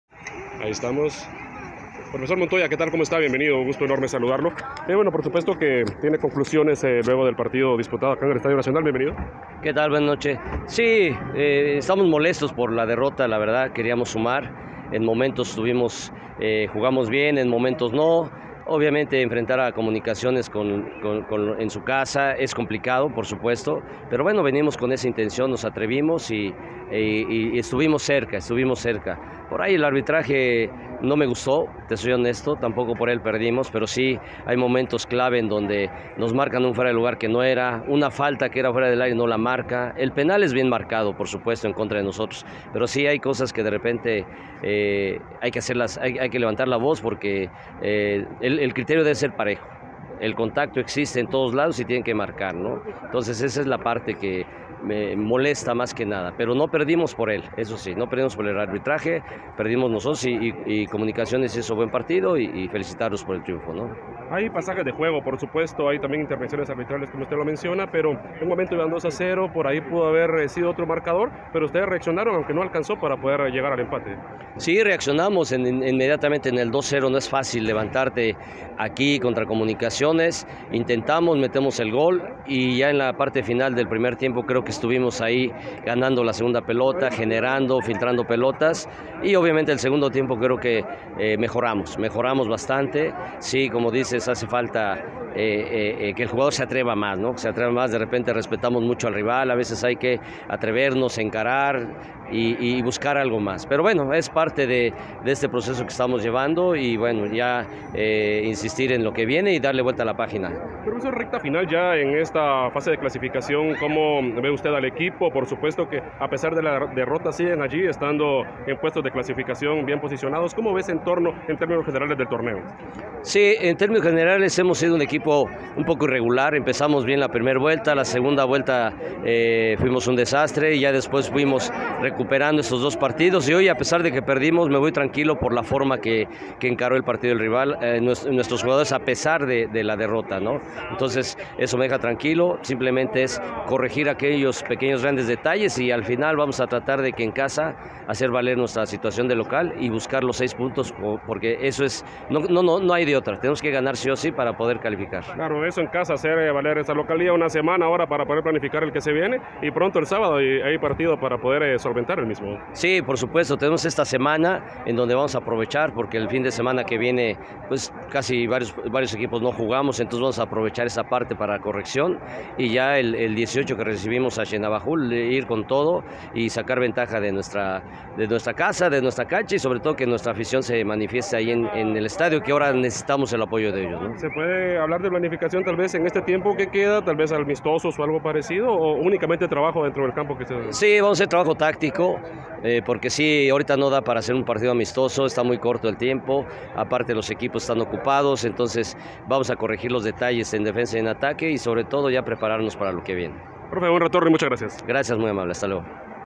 Roberto Montoya - Comunicaciones FC 2-1 Guastatoya
El técnico de los pecho amarillo habla después de finalizado el partido de la Jornada 15 ante los cremas.